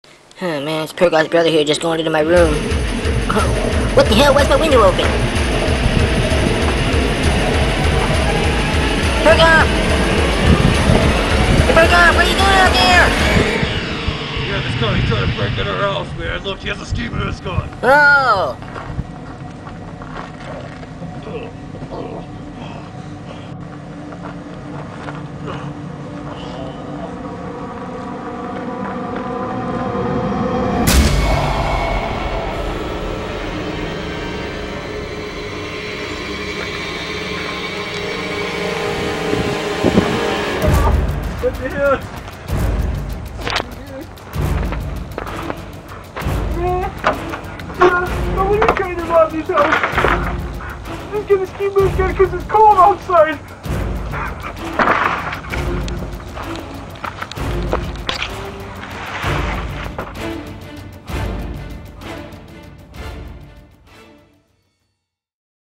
Thanksgiving Day Robbery
I was coming home from my grandmas house and saw a guy in a ski mask standing like right in front of our house.

These robbers need to stop wearing ski masks, it's so obvious what they are up to.